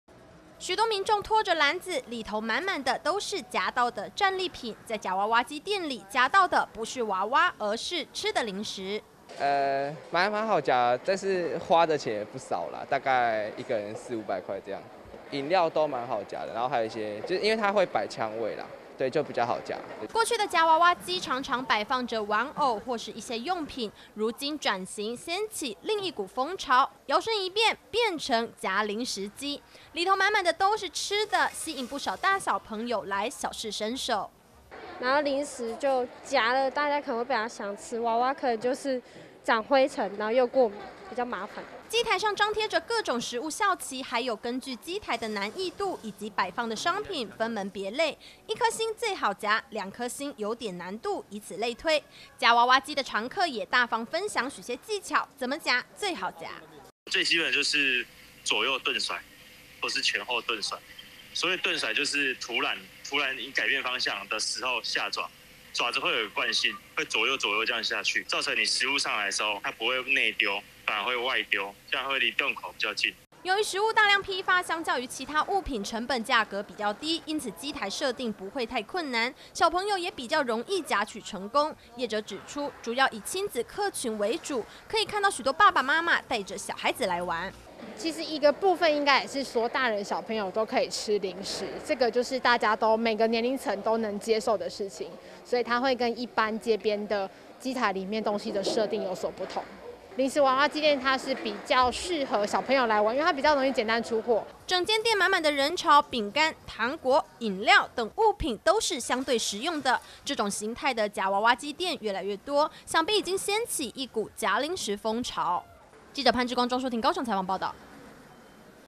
夾吃的| 夾娃娃機拚轉型 夾零食夯！出貨率比較高
過去夾娃娃機內放的東西幾乎都是玩偶，或是一些商品、公仔等等，如今，掀起另一股風潮，許多娃娃機店裡頭，機台內滿滿的都是吃的零食，因為成本相對低，出貨率也比街邊店相對高了點，吸引不少親子客群來小試身手，夾了滿滿的一車零食準備大吃特吃。